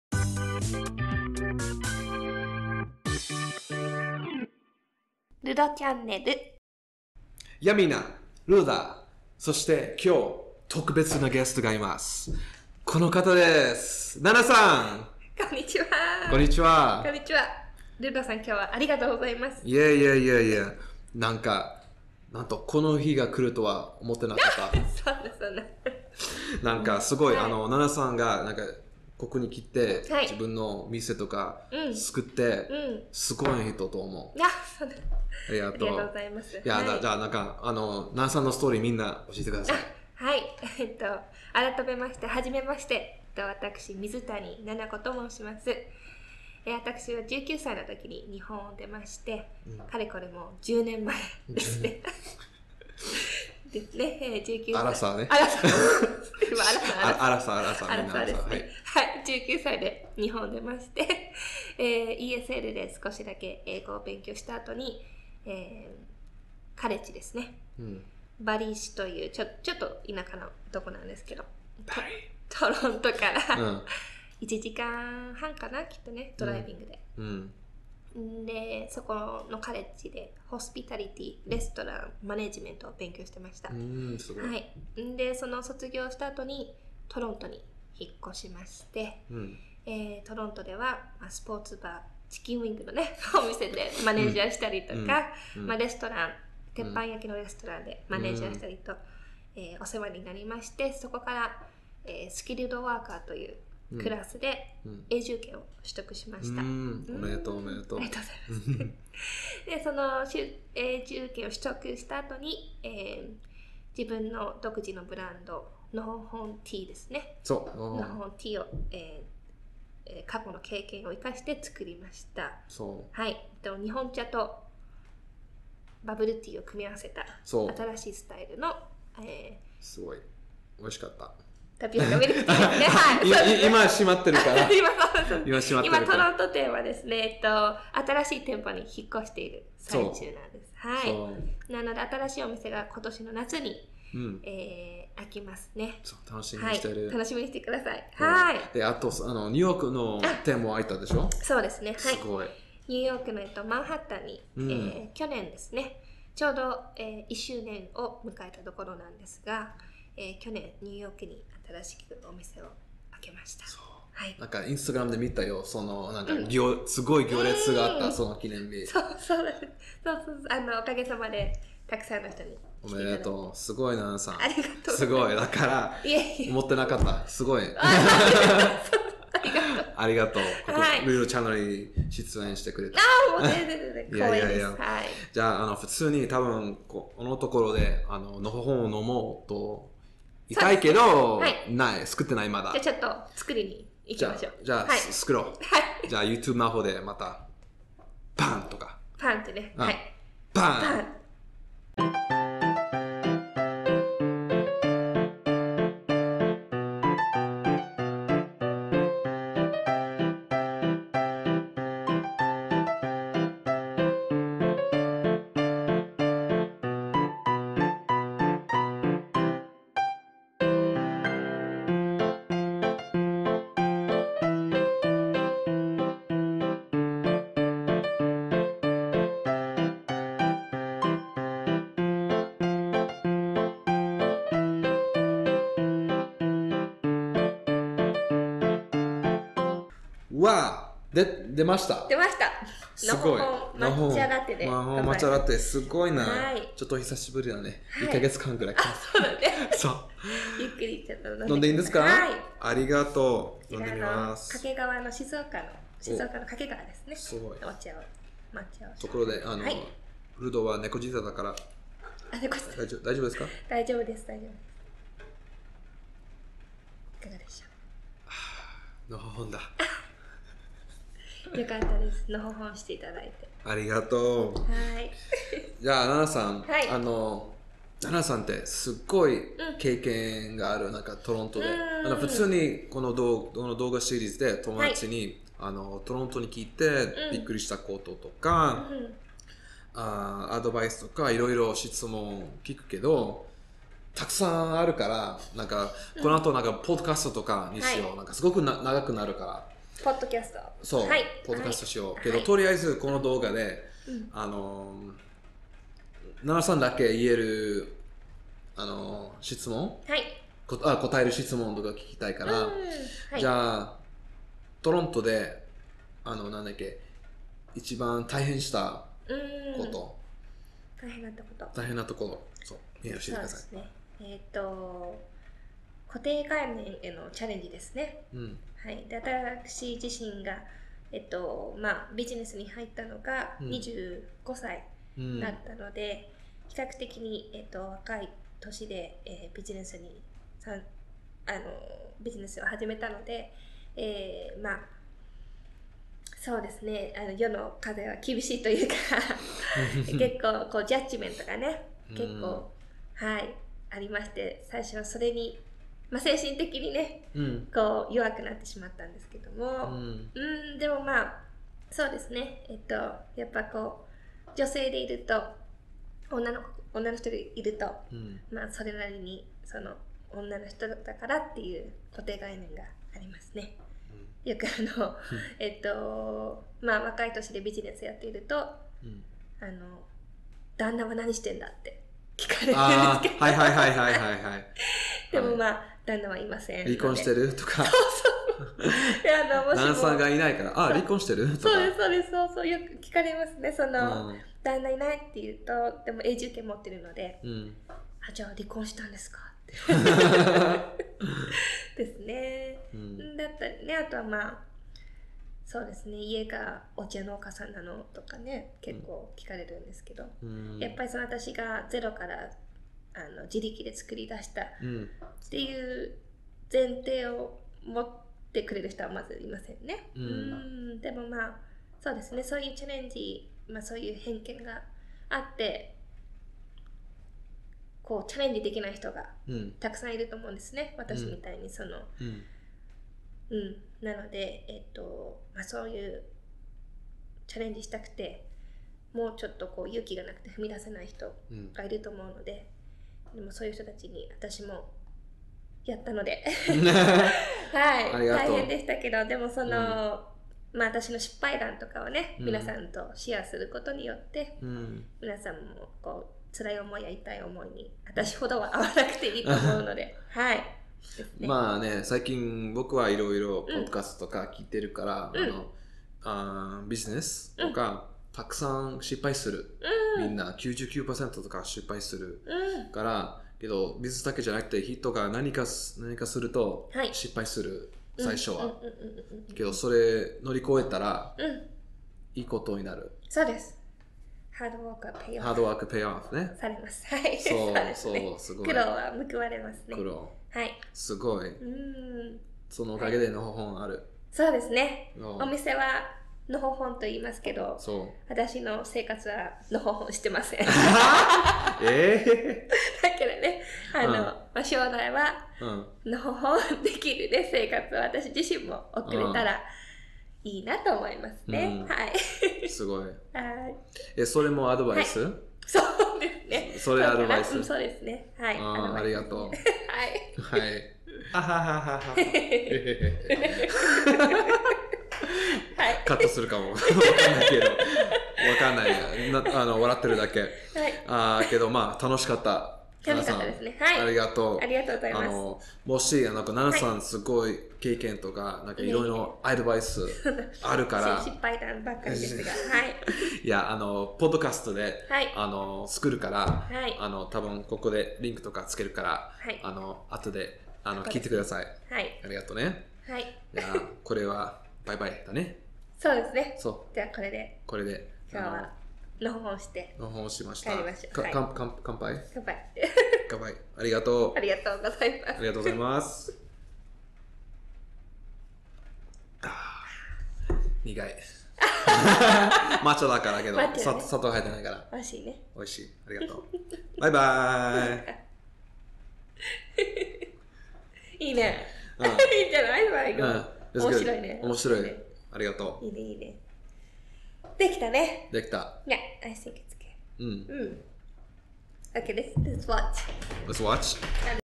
留学生見るべき！トロントで自分の店を開いた Nanako Mizutani「ルドは友達と話す」
PODCAST!!

ななこさんは、のほほんティールームという日本バブルティー(タピオカ)のお店を経営しています。お店では日本から取り寄せた抹茶の粉末を使用してバブルティーを作っています。現在トロントとニューヨークに店舗があります。ななこさんは若い時にカナダに一人できました。そして今まで沢山の壁を乗り越えてきました。

About this video:
Nanako Mizutani is the owner of Nohohon Tea Room, a Japanese-style bubble-tea store specializing in matcha green tea, with locations in Toronto and New York. She came to Canada by herself at a young age, and overcame many obstacles to get to where she is today.

Follow Nanako and Nohohon on Instagram!